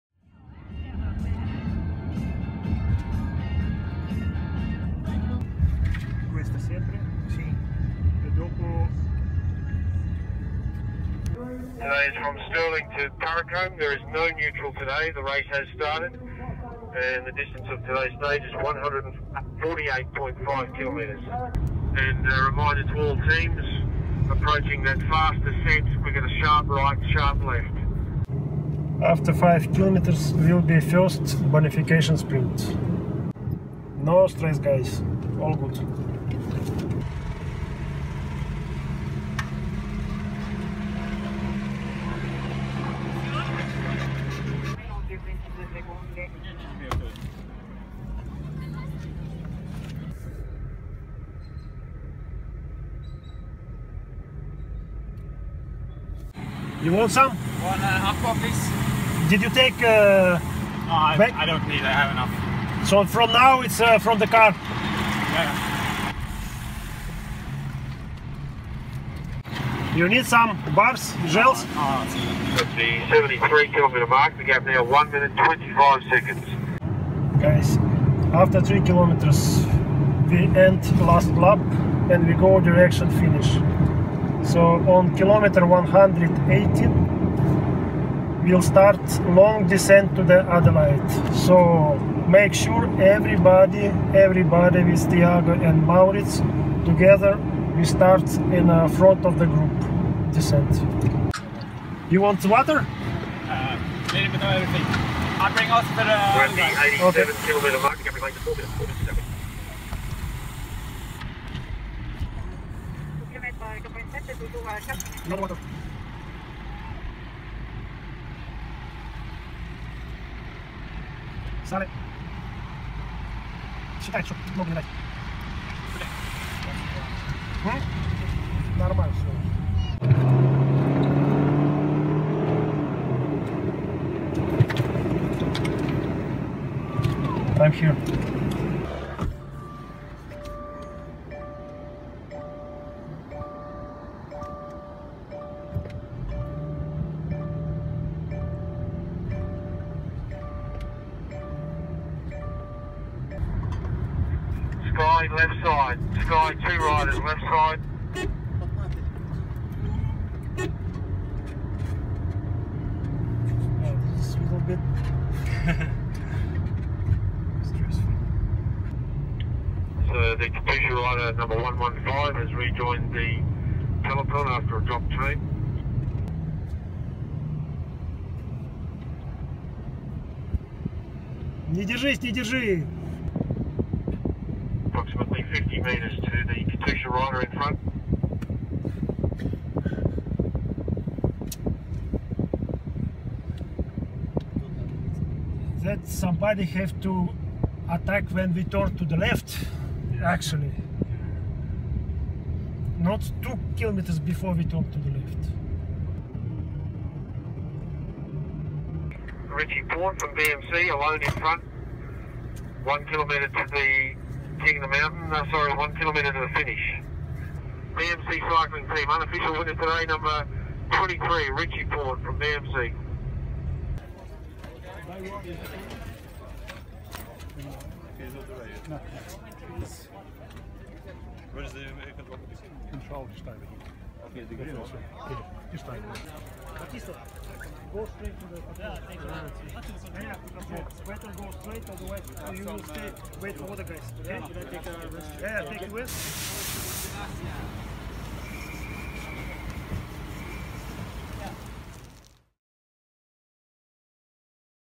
Inside the Tour Down Under convoy with Katusha-Alpecin
Come along for a ride in the convoy at the 2017 Tour Down Under with Team Katusha-Alpecin.